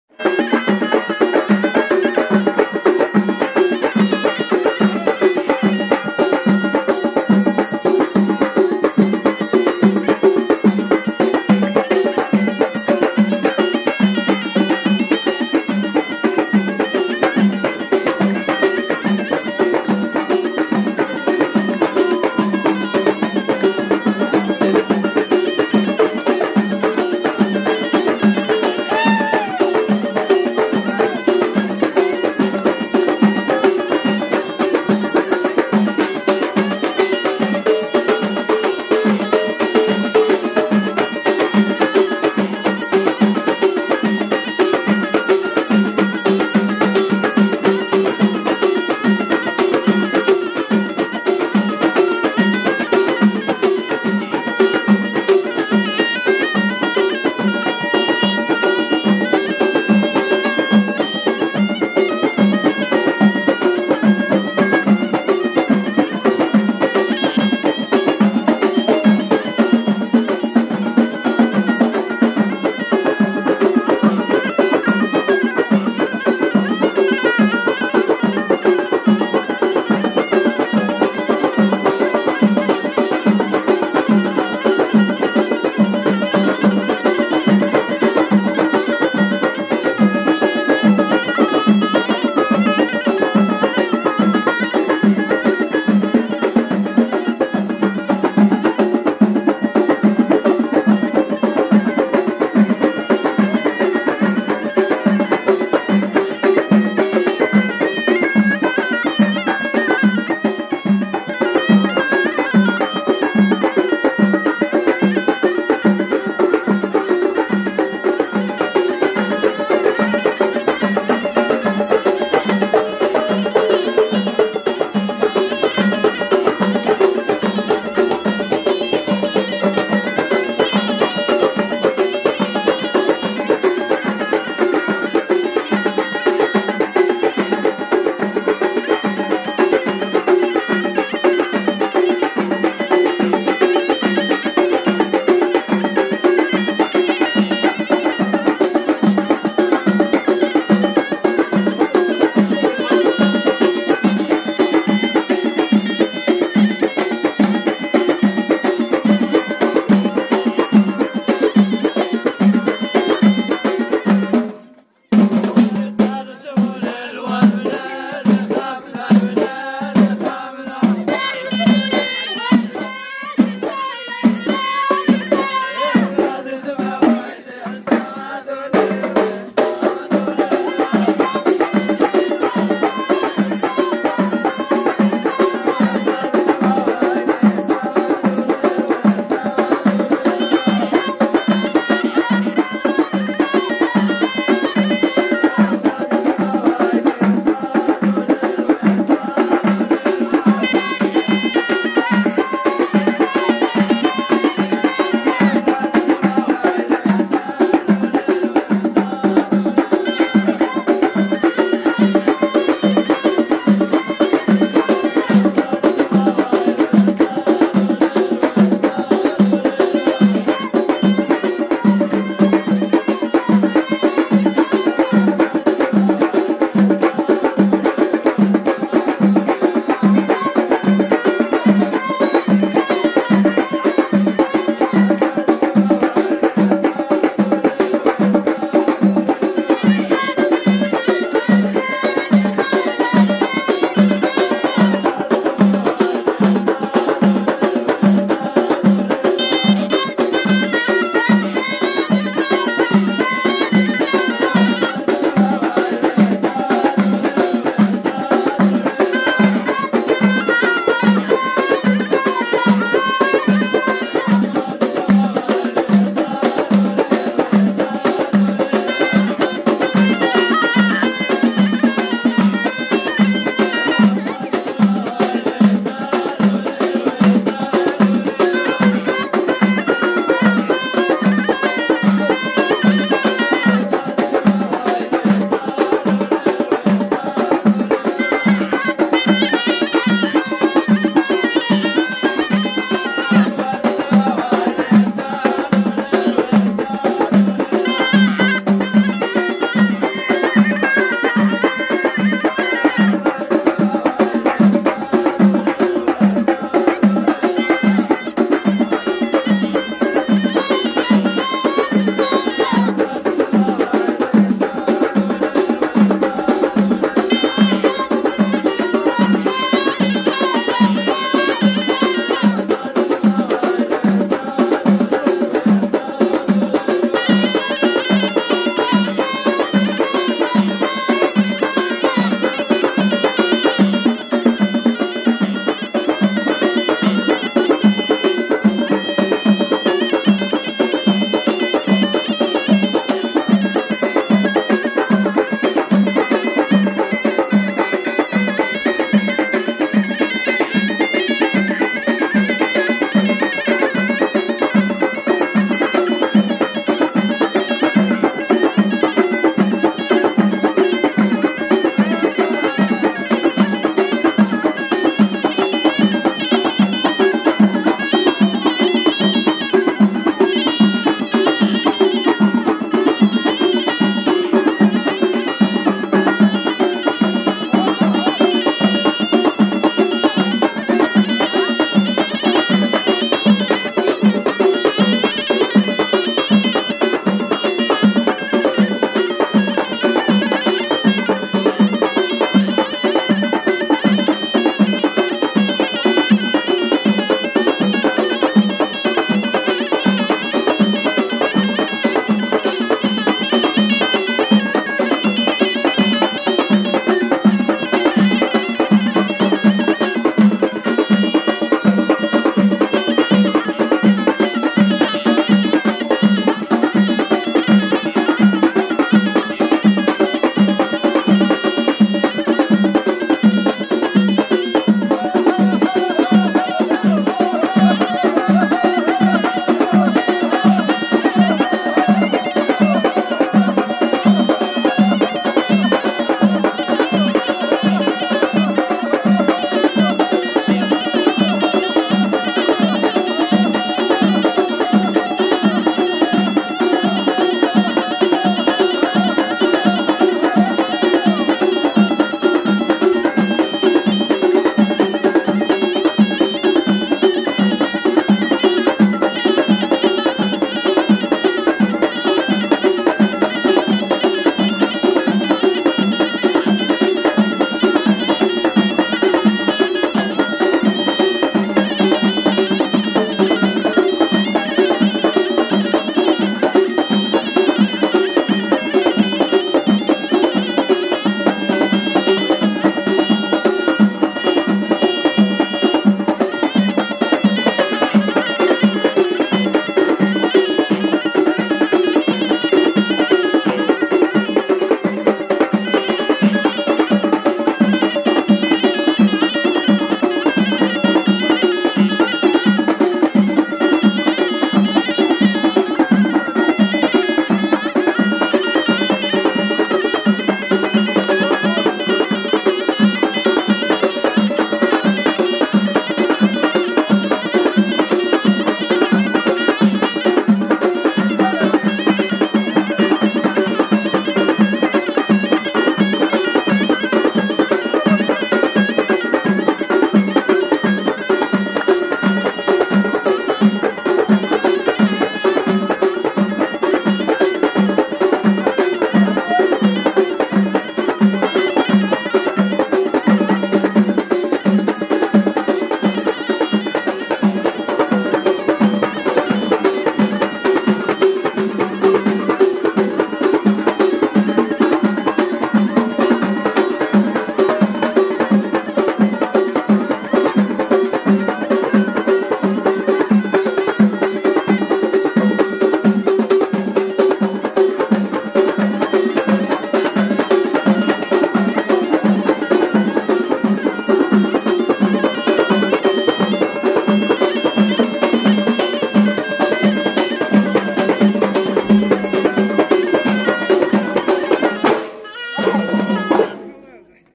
الغيطة toop ghita غربل نايضة الشطيح والرديح من التراث الأمازيغي مع فرقة تمونت لفنون الغيطة
فن من فنون الثقافة الأمازيغية آلة المزمار ..

الغيطة الشعبية
الغيطة الأمازيغية الأطلسية
فرقة تمونت لفنون الغيطة
أغنية أمازيغية رائعة أغاني  الاطلس